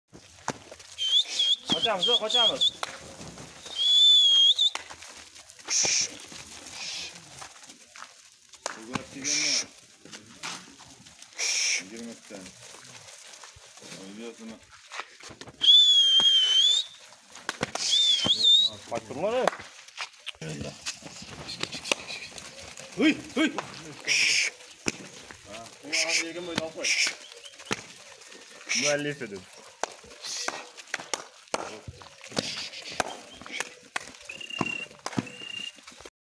Beshkek qoʻzi qoʻchqorlari sotiladi soni 22 dona narxi 2200 dan kelshamiz Tel:330272000
Beshkek qoʻzi qoʻchqorlari sotiladi soni 22 dona yoshi 7yo 8 oy atrofda narxi 2200 dan kelshamiz manzil Namangan toʻraqoʻrgʻon saroy
Dastavka bor